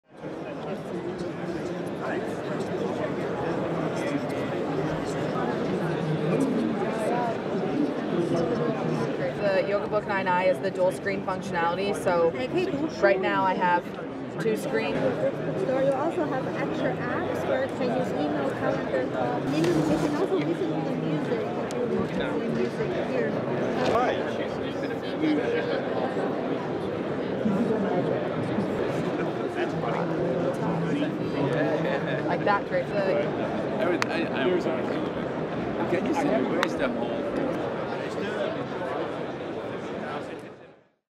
Lenovo showcases foldable devices at MWC
Lenovo displayed the Yoga dual-screen tablet, the latest version of their Thinkpad X1 foldable laptop and a prototype Thinkbook rollable screen laptop at Barcelona's Mobile World Congress.